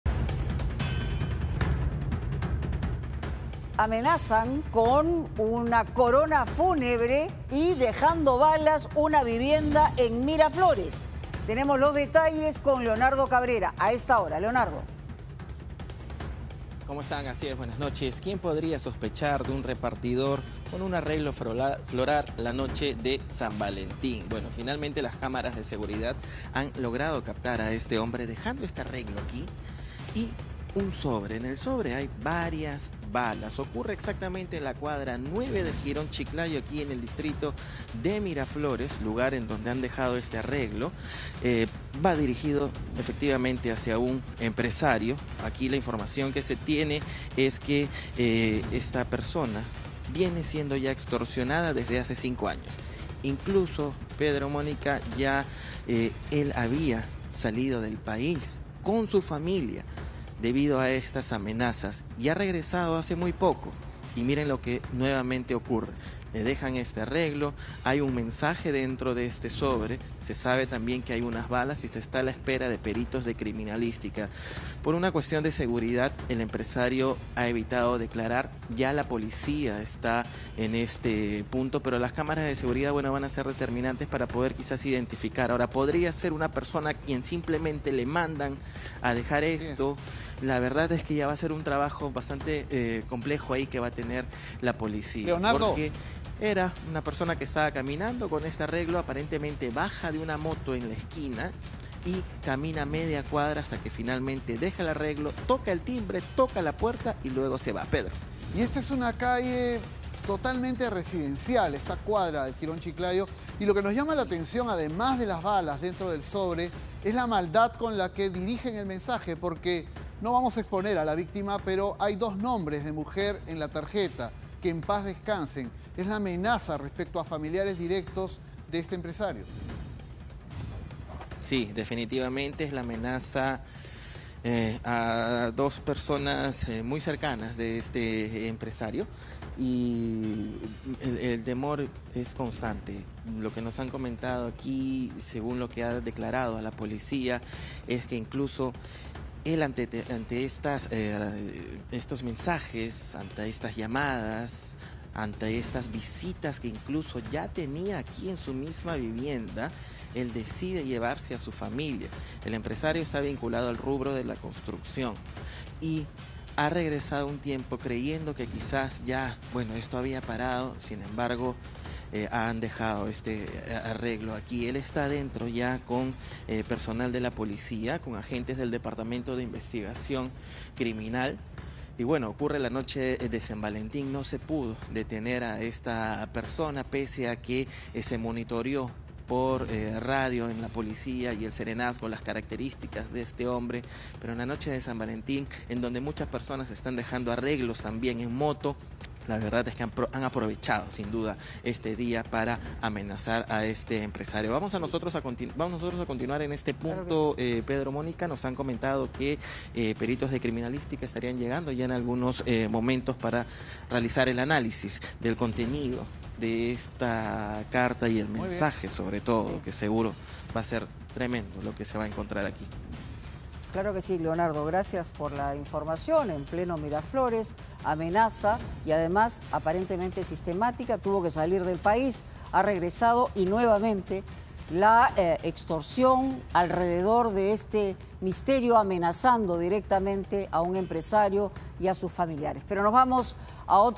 Amenazan con corona fénebre y balas a una vivienda en Miraflores
Amenazan con corona fénebre y balas a vivienda de empresario en Miraflores. La victima venía siendo extorsionada desde hace 5 años y había salido del país.